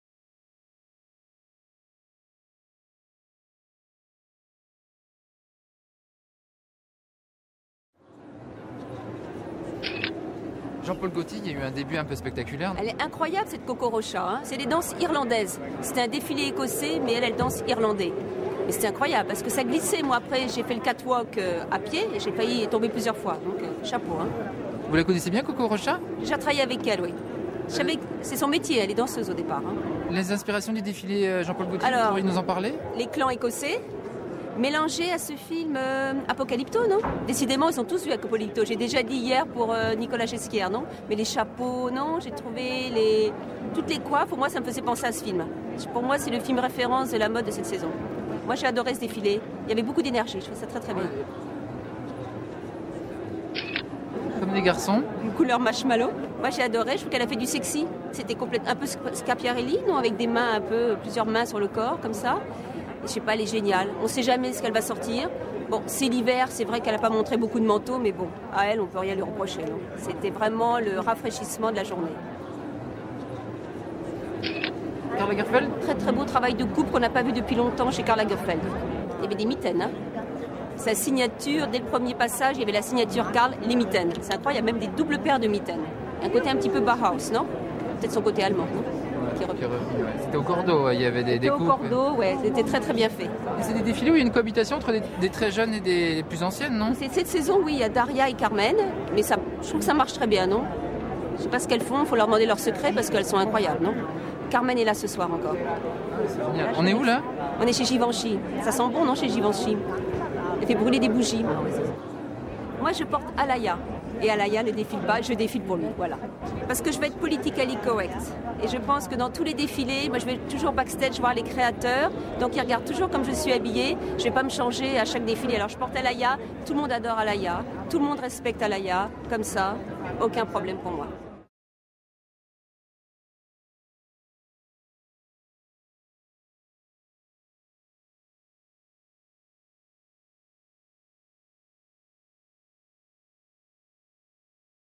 Carine Roitfeld 4
Sur les défilés de Jean-Paul Gaultier, Comme des Garçons et Karl Lagerfeld.

vogue.fr